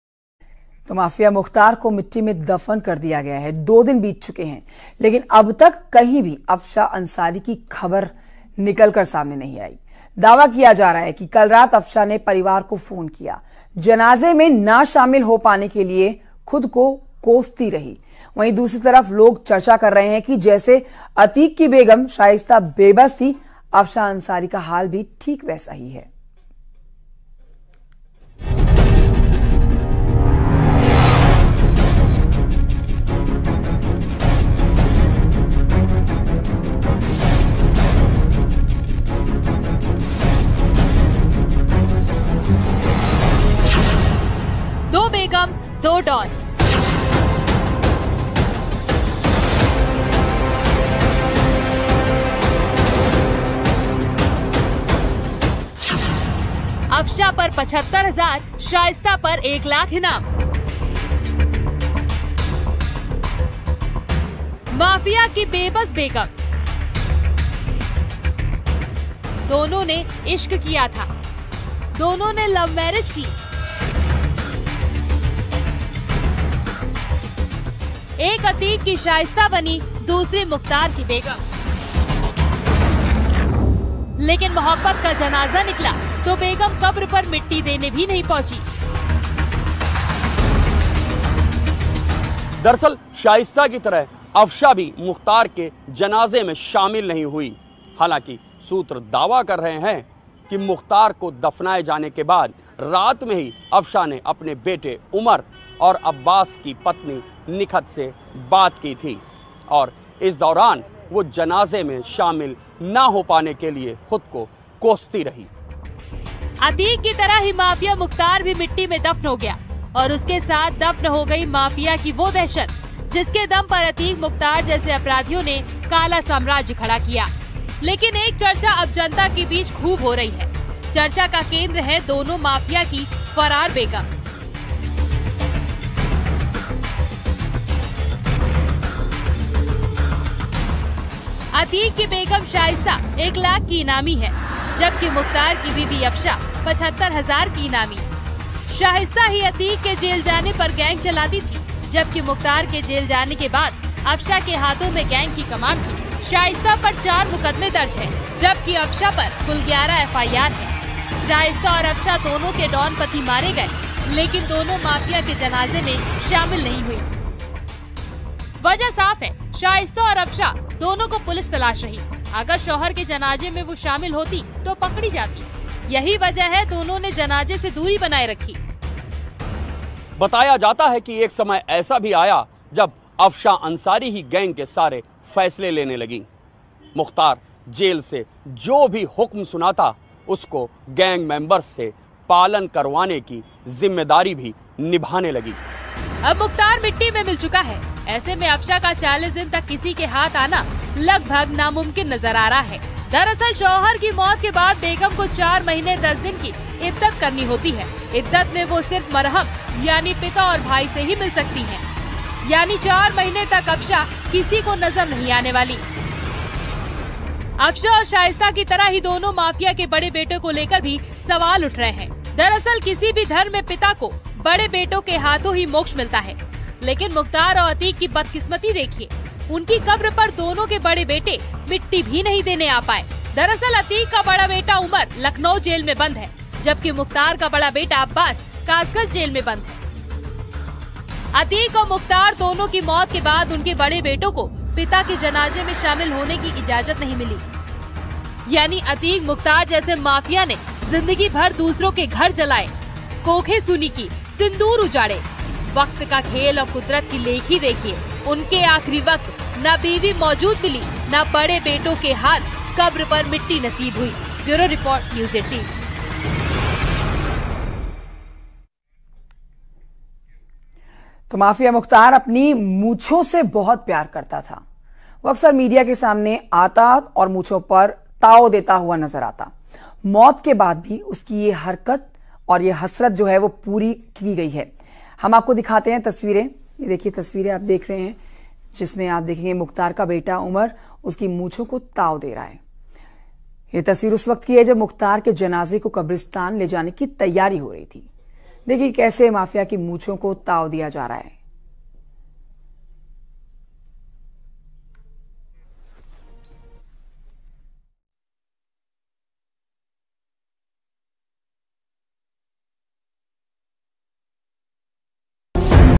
Mukhtar Ansari News : पति मुख्तार अंसारी का जनाज़ा देख रोई पत्नी | UP News | Top News | News18
मुख्तार की पत्नी ने खाई कसम, लेगी बदला ?